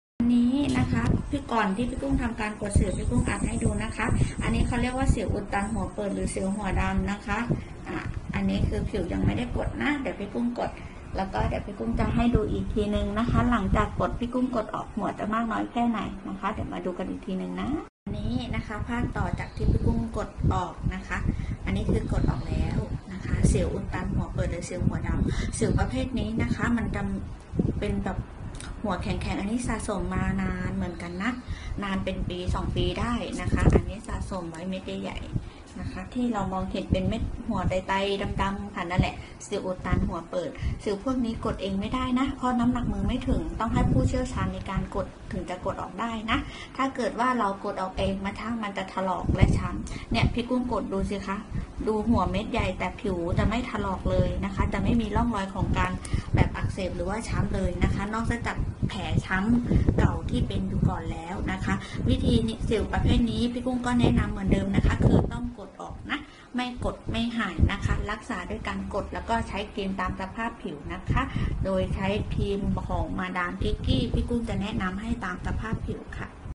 พี่กุ้งกดสิว68
สิวอุดตันหัวเปิด หัวดำใครๆก็บอกว่ากดง่ายพี่กุ้งอยากบอกว่า ยิ่งหัวดำยิ่งลึดและยาวถ้ากดไม่เป็นอักเสบทุกรายค่ะ😍😍และสิวแบบนี้ไม่กดออกจะกลายเห็นหัวแข็งๆๆและกลายเป็นหินได้ค่ะ..รีบๆรักษาน้าอย่าปล่อยไว้นานน้า